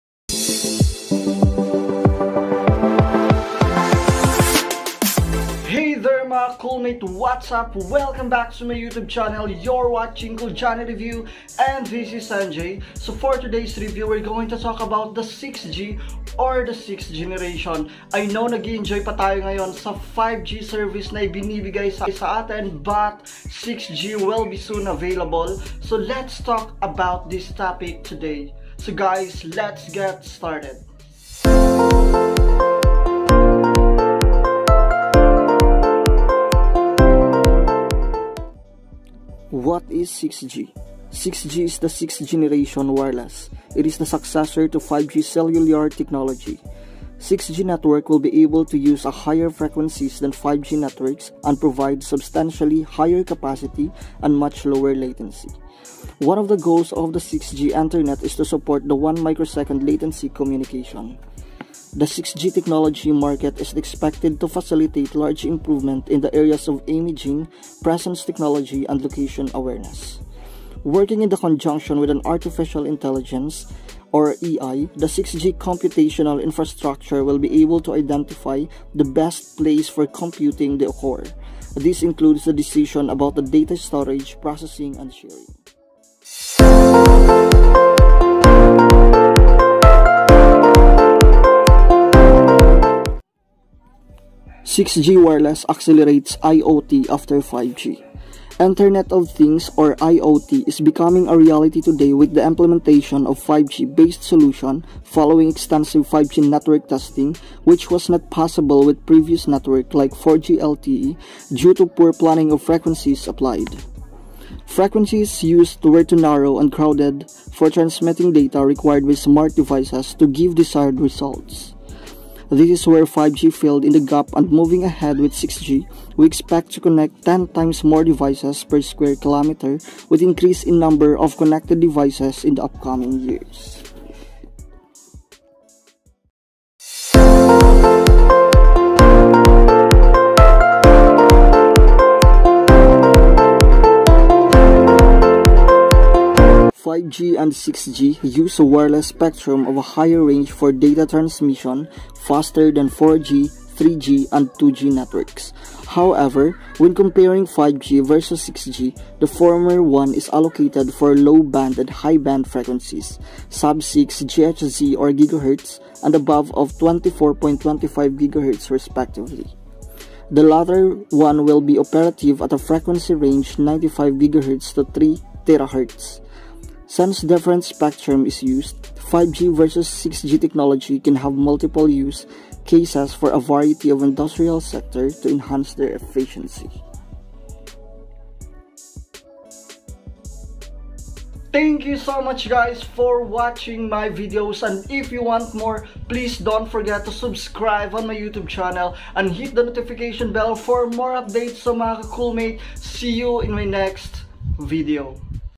6G ( 6th Generation ) Era of IOT
6G will be soon available in 2030 making it more advance than 5G network. In this generation IOT or Internet of things is possible. 6G will be expected to be 100 times faster than 5G with enhanced reliability and wider network coverage.